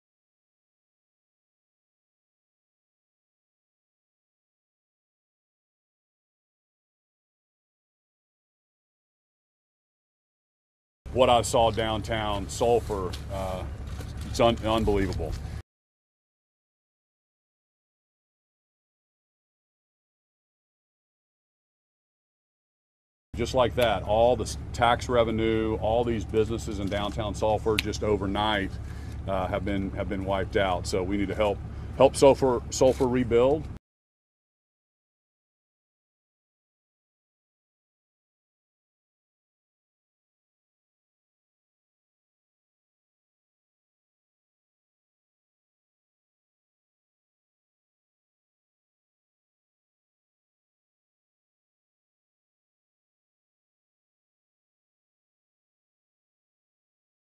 Tornadoes kill 4 in Oklahoma, leaving trail of destruction
Tornadoes killed four people in Oklahoma, including an infant, and left thousands without power Sunday after a destructive outbreak of severe weather flattened buildings in the heart of one rural town and injured at least 100 people across the state.